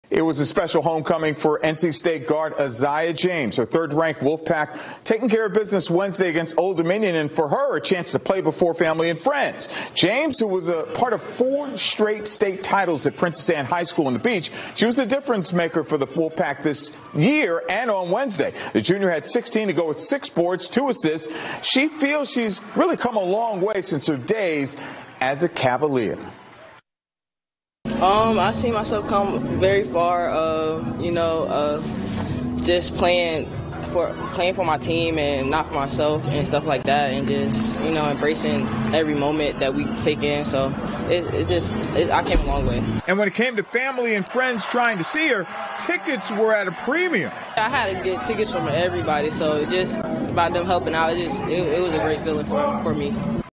N.C. State guard Aziaha James
The Virginia Beach native guided Princess Anne High School to four state titles in basketball.